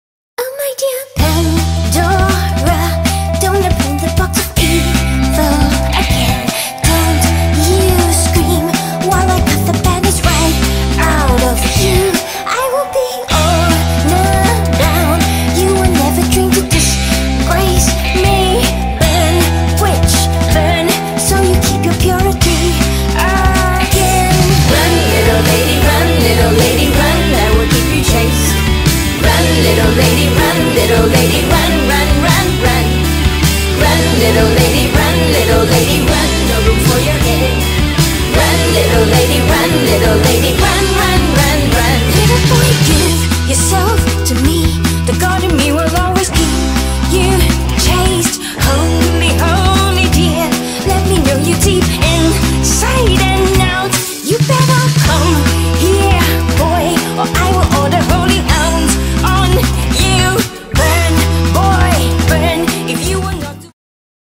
Angel Arias Book Trailer
The book trailer for Angel Arias (bk 2 Night Creatures trilogy) by Marianne de Pierres.